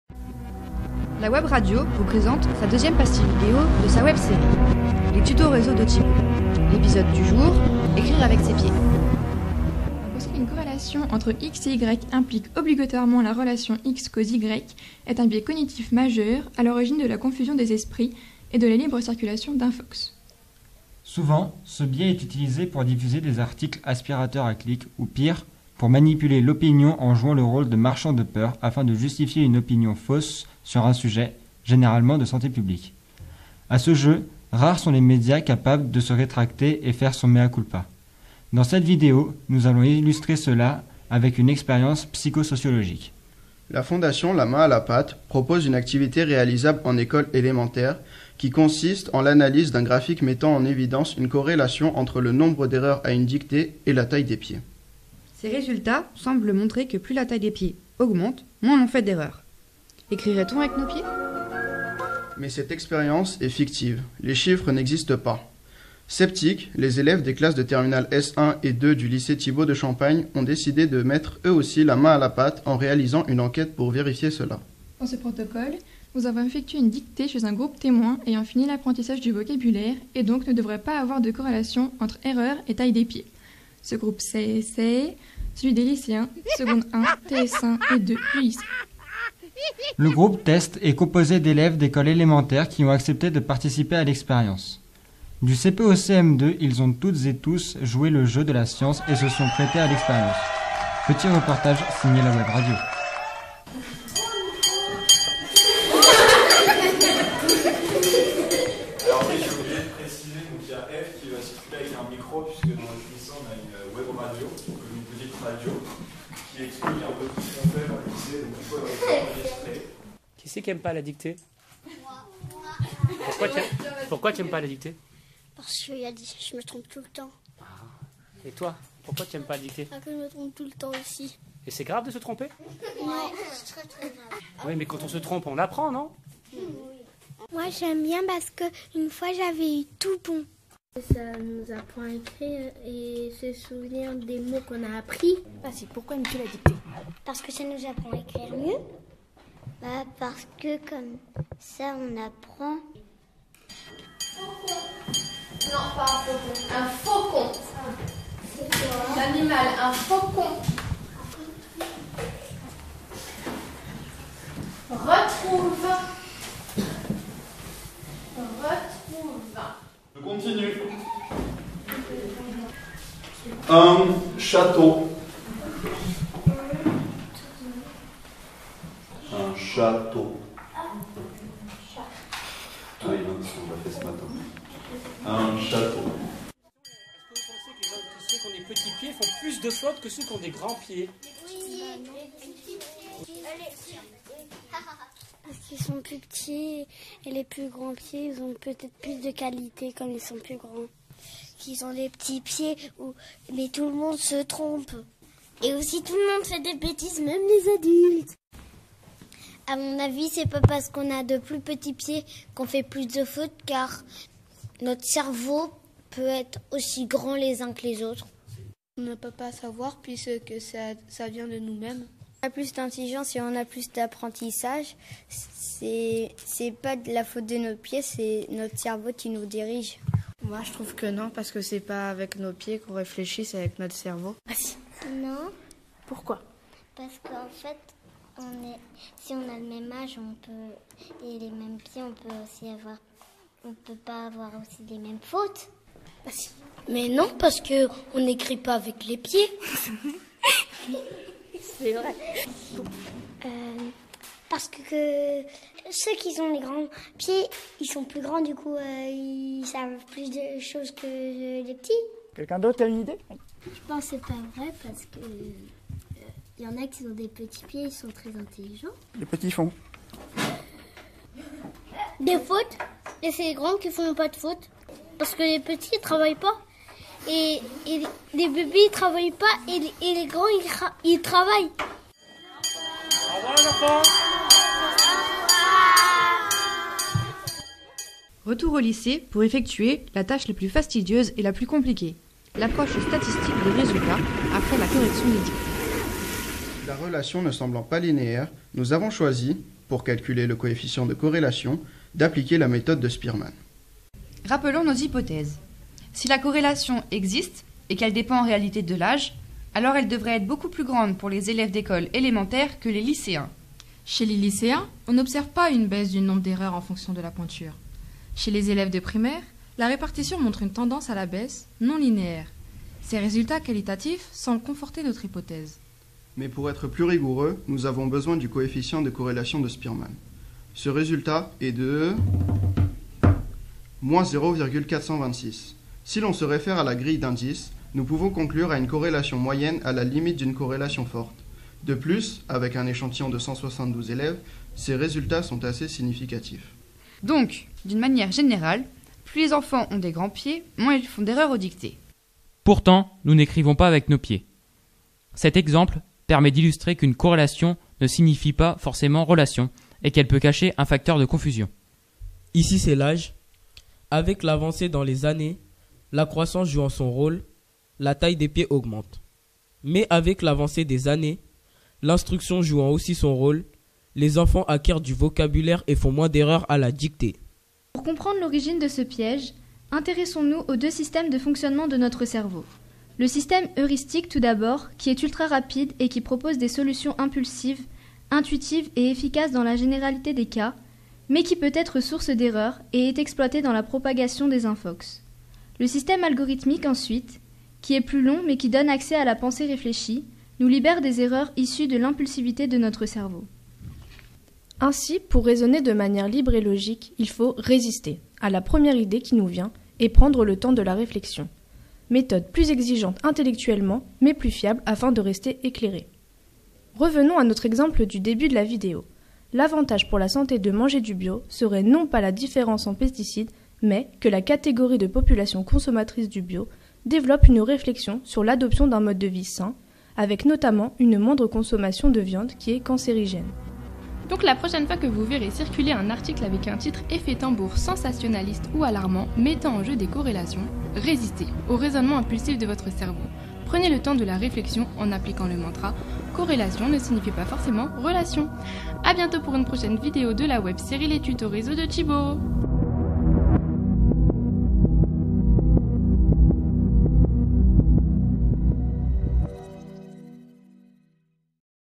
Épisode 2 : Corrélation VS Relation Projet Dictée
Vidéo réalisée dans le cadre des ateliers d'initiation à la Pensée Critique en AP SVT et en collaboration avec la WebRadio.